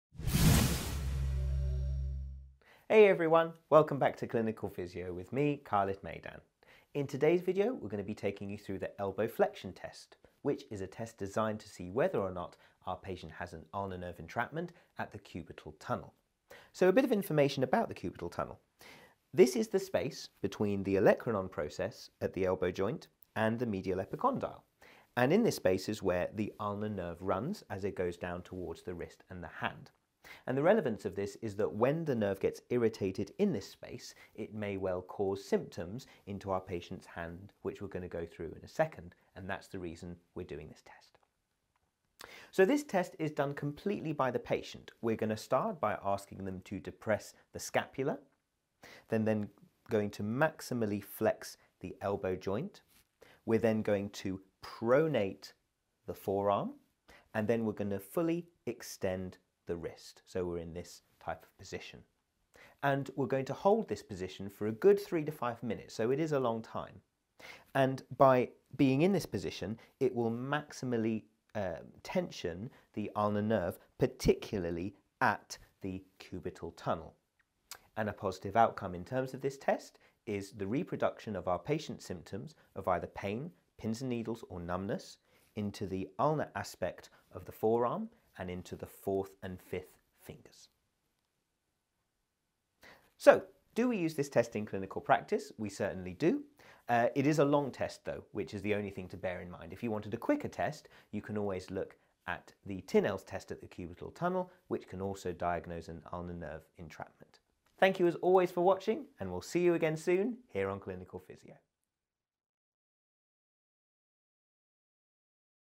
Active Flexion Test for Elbow Cubital Tunnel Syndrome (Ulnar Nerve Entrapment)
This tutorial takes you through this great test that you can use to diagnose your patients with an Ulnar Nerve Entrapment! It teaches you the methodology behind the test, and how to interpret your findings!

If you want to see more of these images head over to our Openstax partners